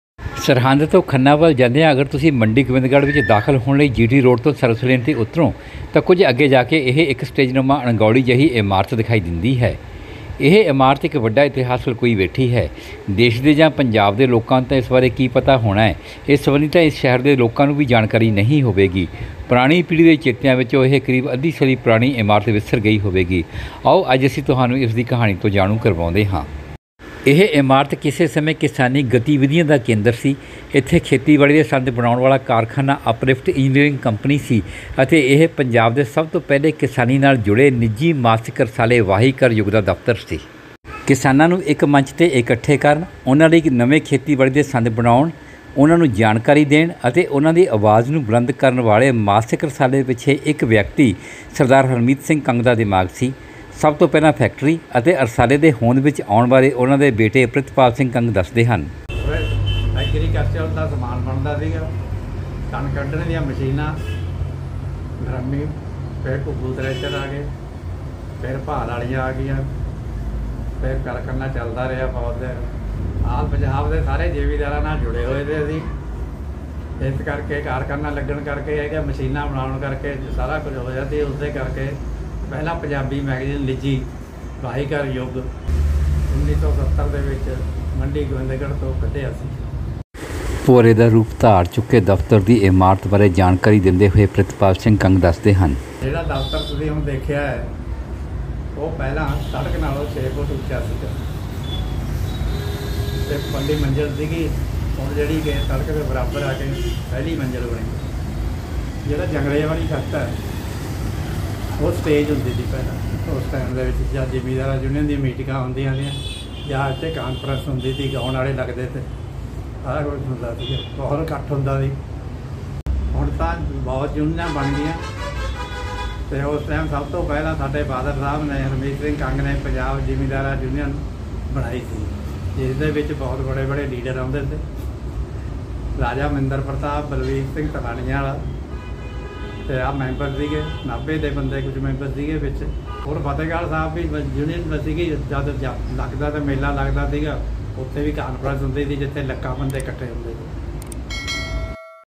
ਸਮੇਂ ਦੀ ਧੂੜ ਹੇਠ ਗਵਾਚੀ ਇਸ ਇਮਾਰਤ ਦੀ ਕਹਾਣੀ । समय के धूल में खोई इस इमारत की कहानी
ਮੰਡੀ ਗੋਬਿੰਦਗੜ੍ਹ ਦੀ ਕਦੇ ਕਿਸਾਨੀ ਸੰਘਰਸ਼, ਪੱਤਰਕਾਰੀ ਅਤੇ ਖੇਤੀ ਉਦਯੋਗ ਕਰਨ ਚਰਚਾ ਵਿੱਚ ਰਹੀ ਇਮਾਰਤ ਹੁਣ ਸਮੇਂ ਦੀ ਧੂੜ ਹੇਠ ਗਵਾਚ ਗਈ ਹੈ। ਆਓ ਜਾਣਦੇ ਹਾਂ ਇਸ ਬਾਰੇ। @oncamerapunjab